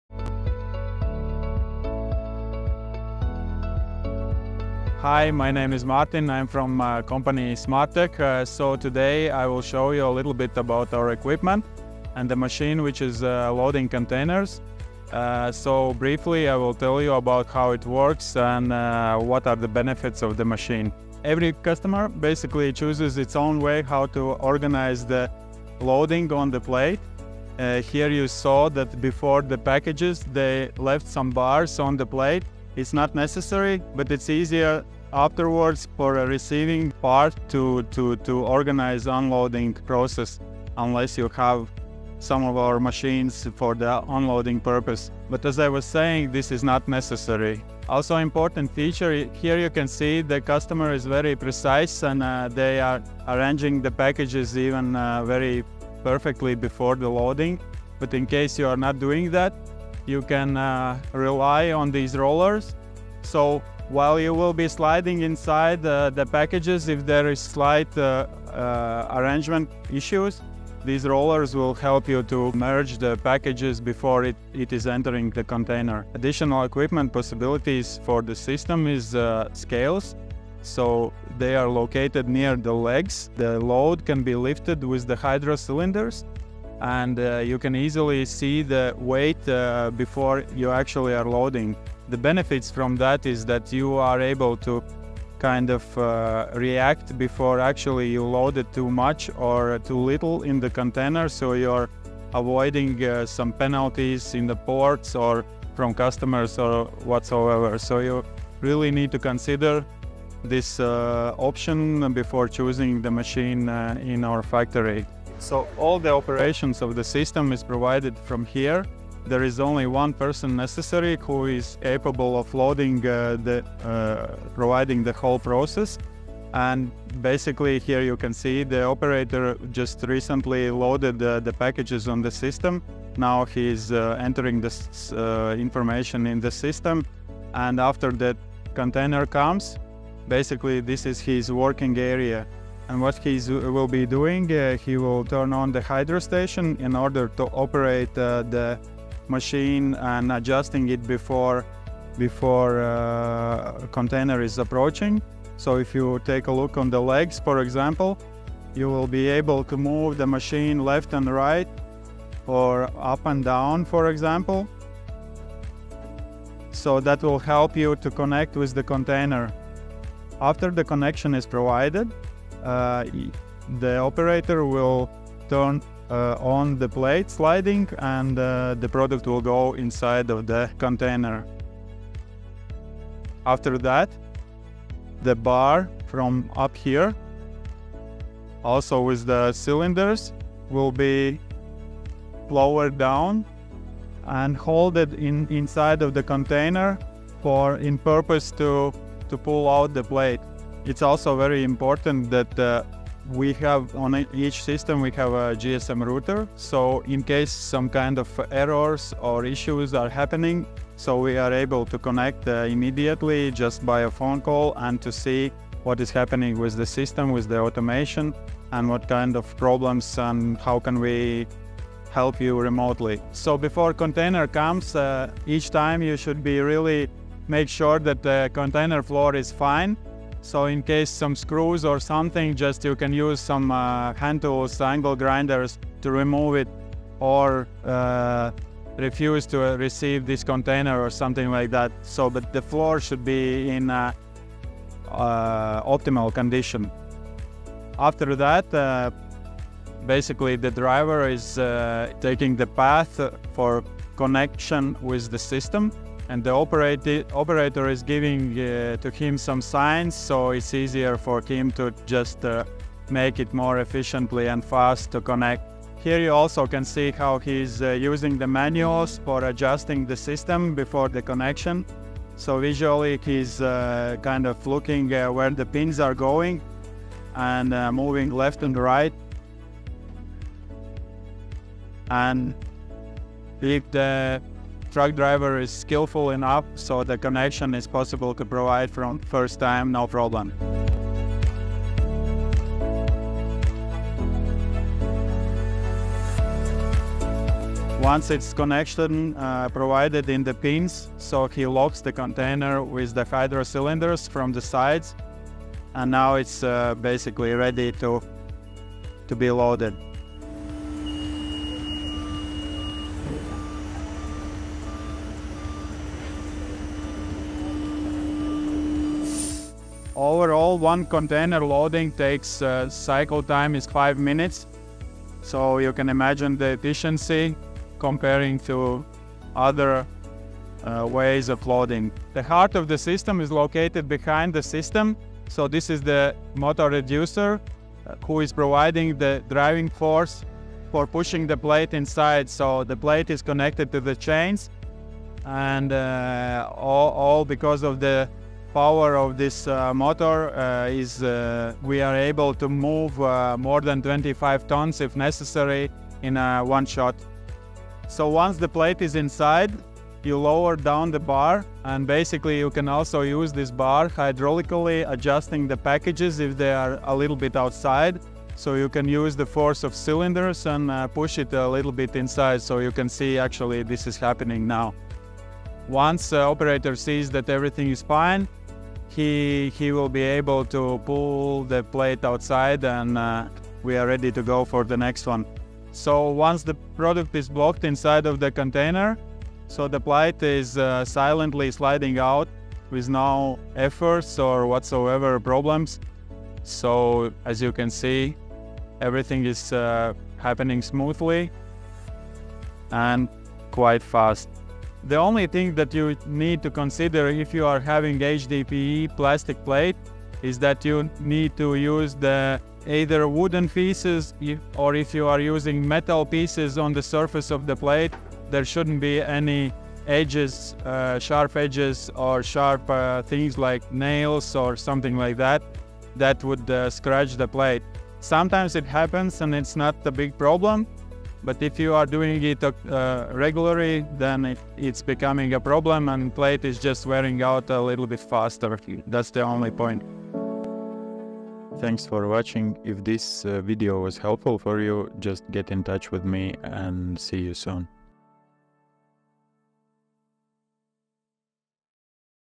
Exploring Automated Container Loading: A Detailed Walkthrough
🚀 Join Mārtiņš, CEO of SmartTEH as he introduces the groundbreaking Automatic Container Loading System. This video showcases the system’s unique features such as remote troubleshooting via a GSM router, a one-operator loading process, and the integration of scales for pre-loading weighing. Witness firsthand how this innovative technology streamlines container loading, enhancing safety and efficiency in warehouse logistics. 📦 Dive into the details and operational benefits of this cutting-edge solution.

Why Watch?
🤔 Understand the latest in container loading technology.
🌟 Learn how to optimize your logistics operations with minimal effort.

0:00-0:20 Meet Mārtiņš: SmartTEH
0:20-1:15 Flexible Options for System Loading
1:15-1:55 Precise Weighing with Scales
1:55-3:20 One-Person Operation: System Controls Guide
3:20-3:43 GSM Router for Instant Support
3:43-5:10 Efficient Container Connection
5:10-5:32 5-Minute Efficient Loading Cycle
5:32-5:58 The Power Behind Loading
5:58-6:45 Plate Mechanism: Loading Done
6:45-7:20 Maintaining the HDPE Plate
7:20-7:32 Thank You for watching